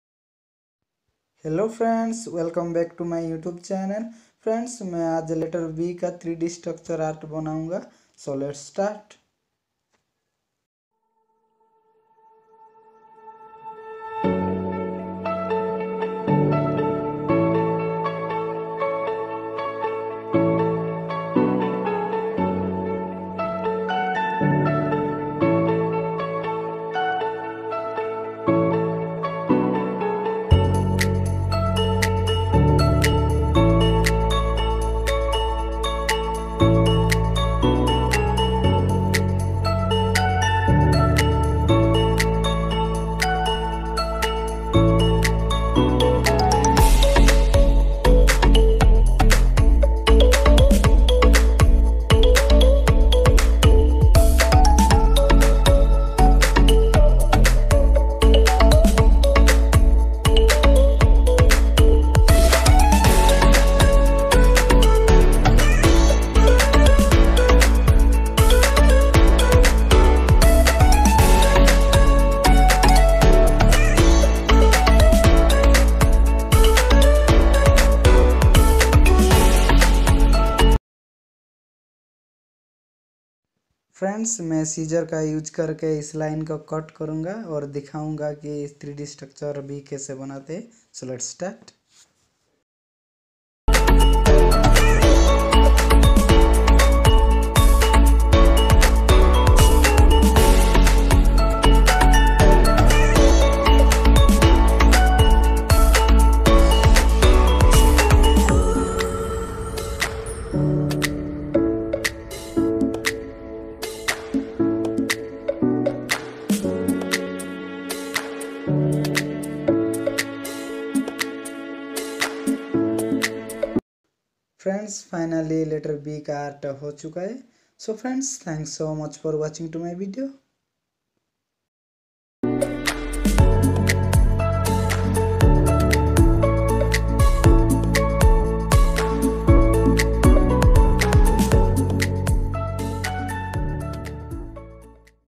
3D art Letter "B".easy process ???
Hello
         Friends aj main letter"B"ka3D art banaunga..

Easy process ...

2020 3D art of letter "B".

I hope ki all of you enjoy this video..

Thanks for lots of love..❤❤❤❤❤